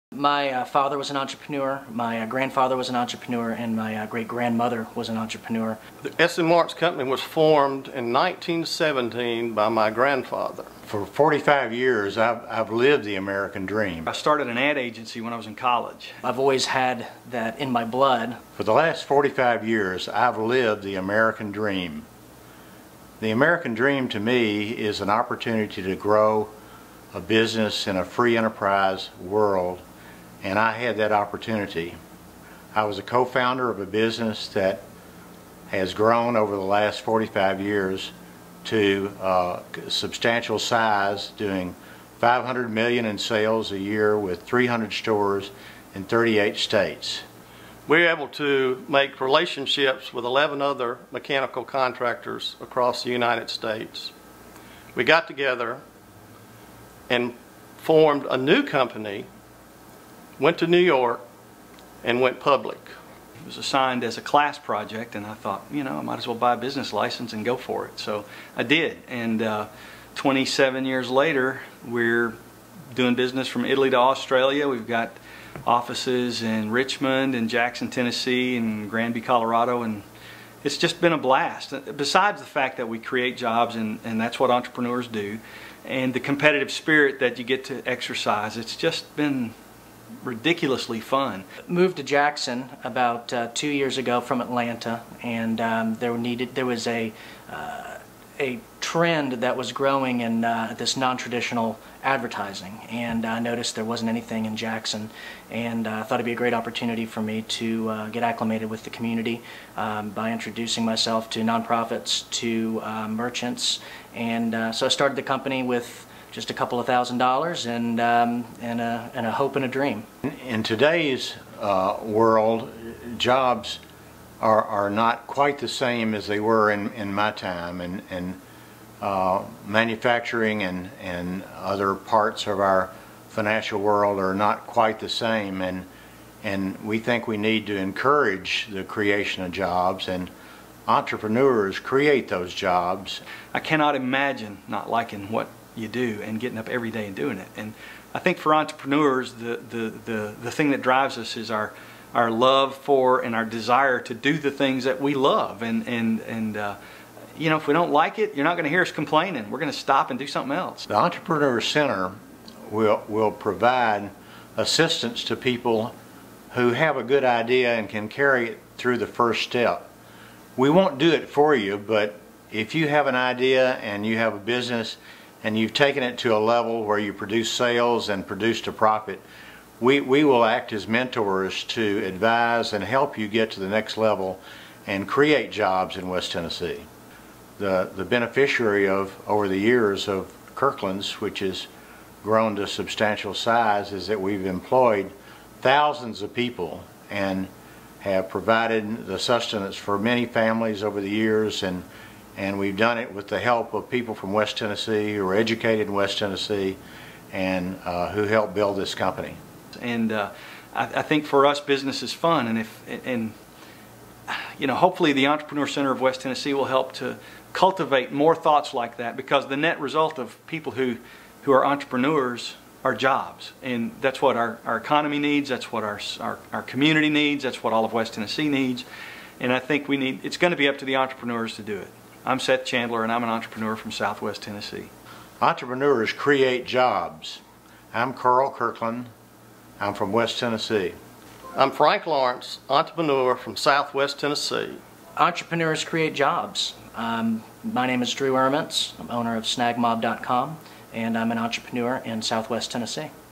Entrepreneur Development Center - Promotional
The Entrepreneur Development Center is a group of experienced entrepreneurs available to mentor up-and-coming entrepreneurs